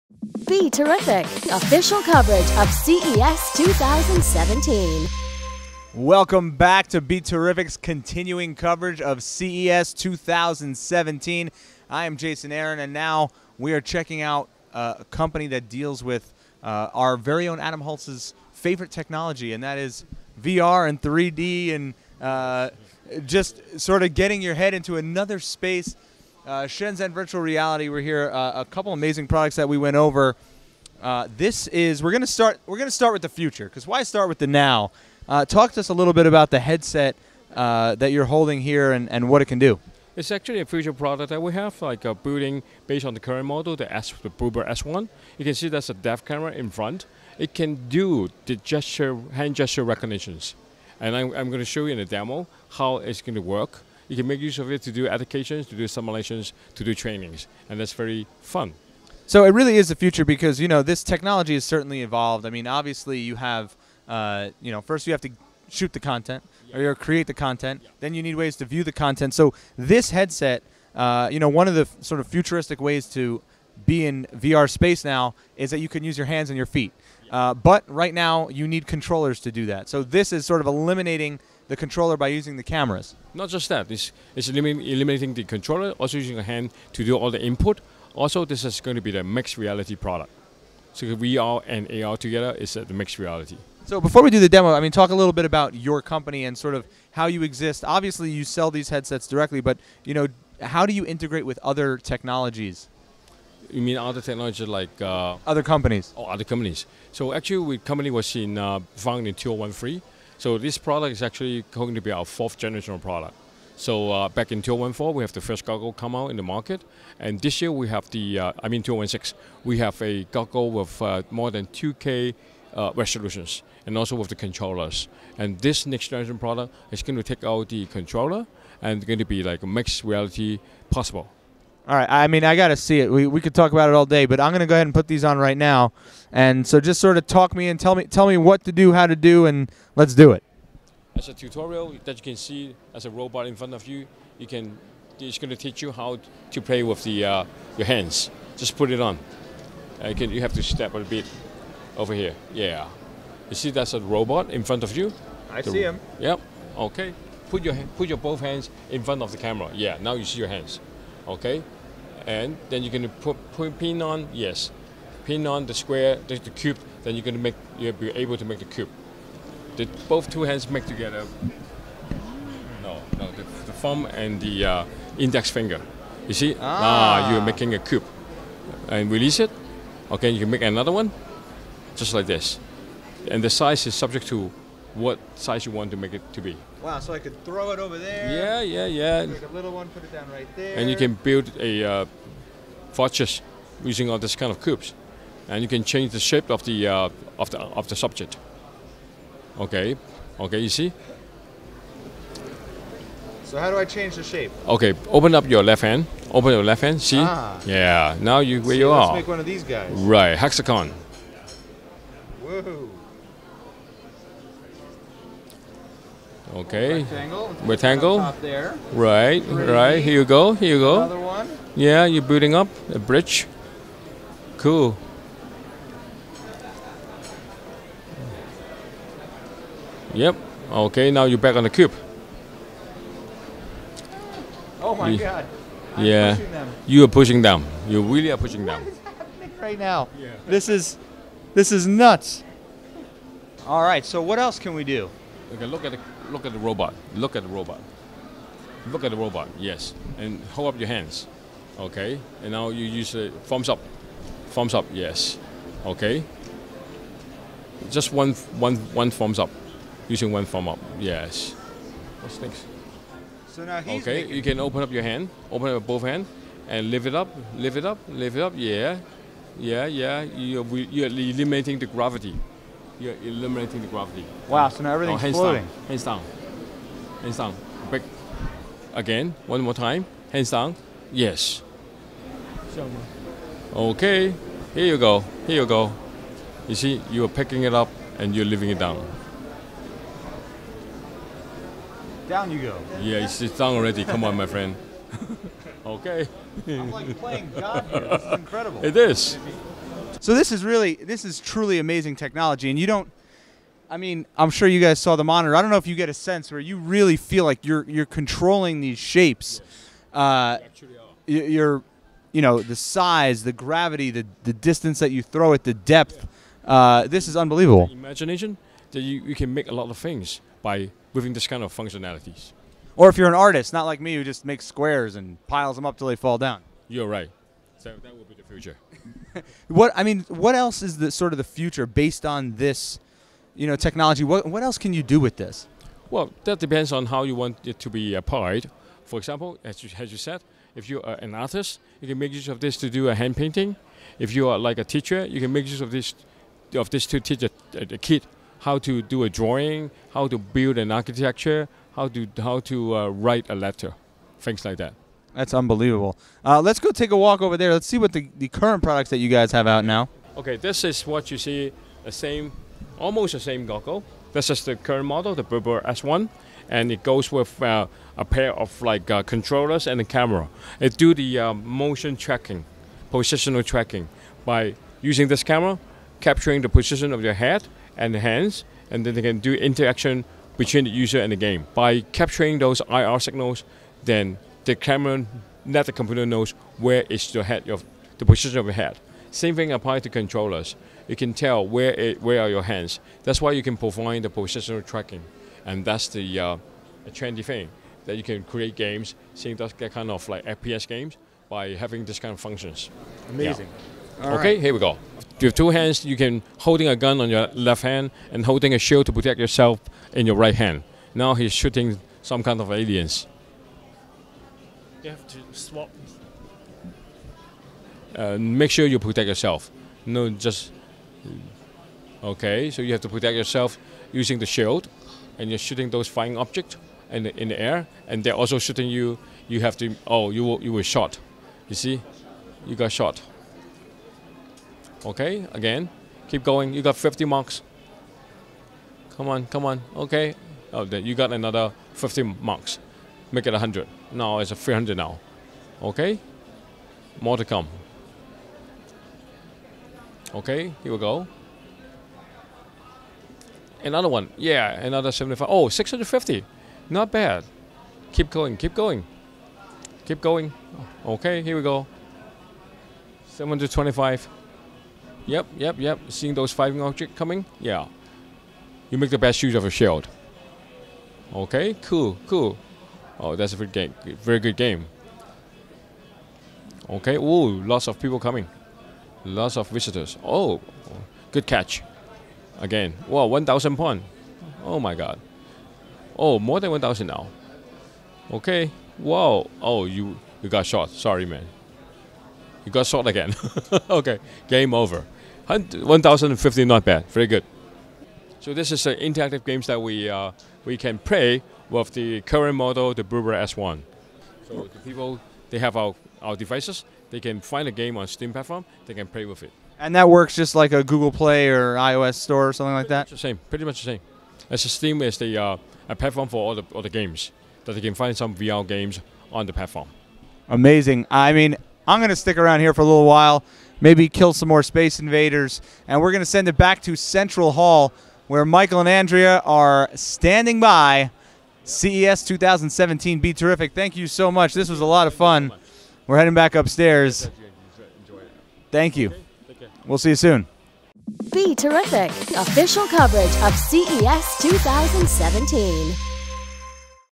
3Glasses Mixed Reality VR/AR Headset at CES 2017 on BeTerrific!!
BeTerrific Live from CES 2017!

Jason Aron demoes the upcoming version of 3Glasses' Blubur VR headset that allows for mixed reality and hand gesture control, and plays with the Blubur S1 at CES 2017!